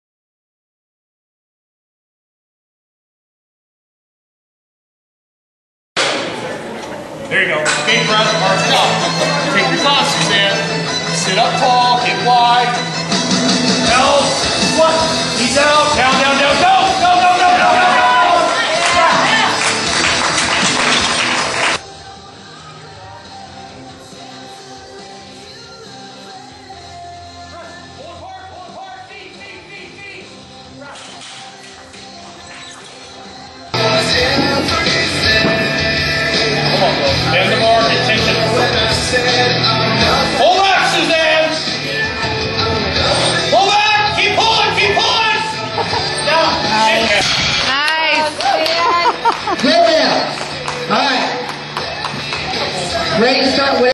my first powerlifting meet
I am a 41 year old mother of two competing in my first powerlifting meet. I started lifting in April 2012 and competed in the NASA KY Regional meet in October 2012.